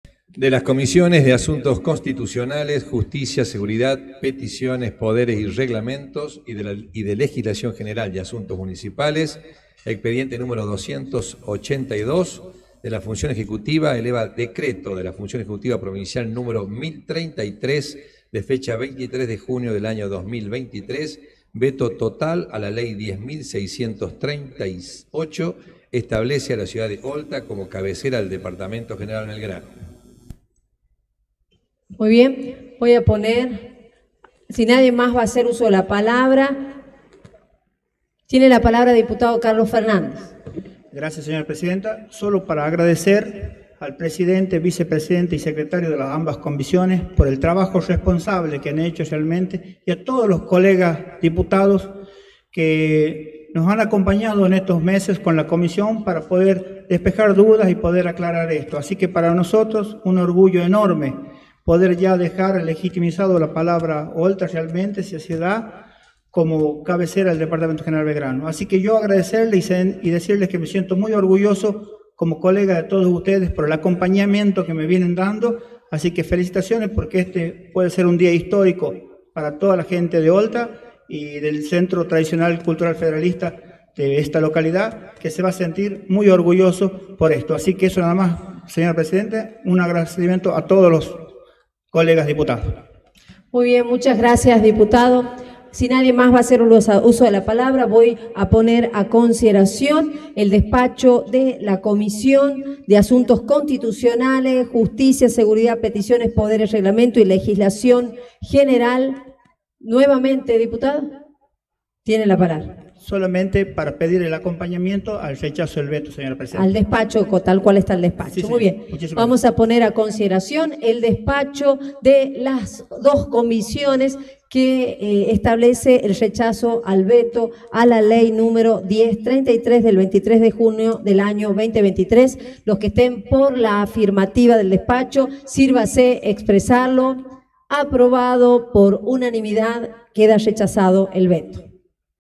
Rechazan veto por Olta
Histórico. Legislatura rechaza un veto del Gobernador y declara oficialmente a Olta como cabecera del departamento General Belgrano.